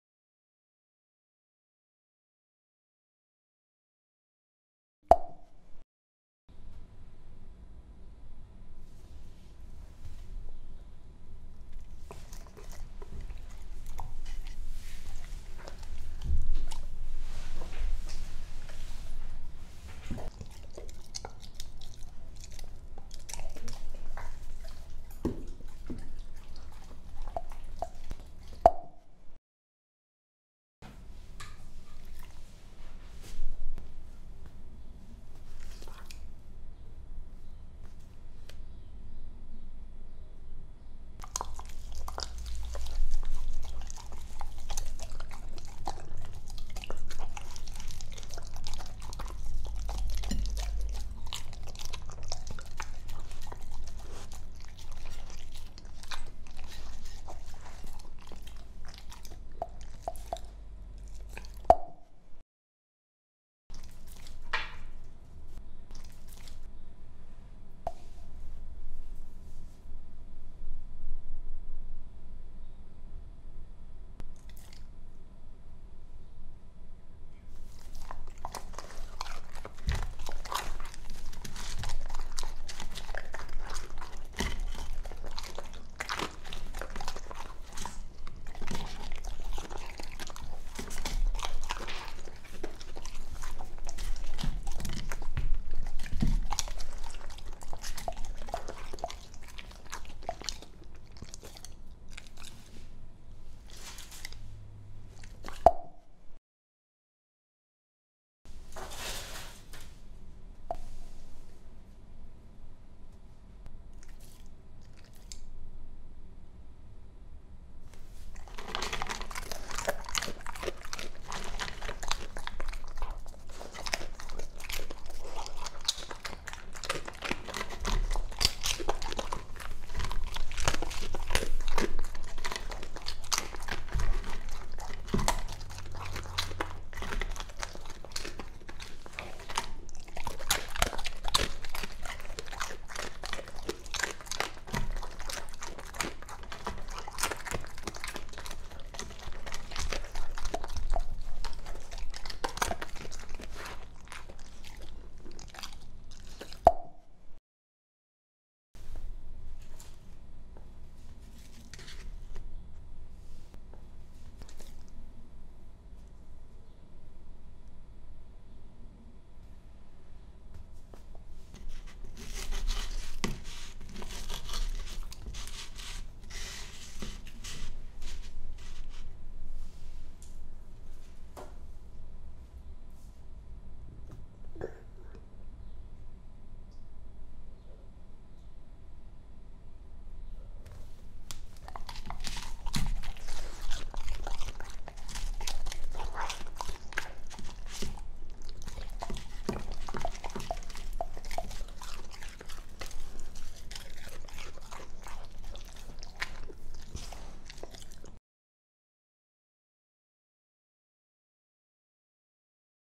ASMR Snack&Smack with Louis - Episode 3 Potatoes
Came from the Bulgarian streets to delight the world with my snacking skills.

This is the third episode of my Snack&Smack ASMR Food Show, there's plenty to follow. Little adjustments here and there, we are improving every single time, enjoy the video!

Please be aware that every single dog is different and therefore requires a different diet. If you are having trouble finding the right nutrition, I would recommend consulting your trusted veterinarian.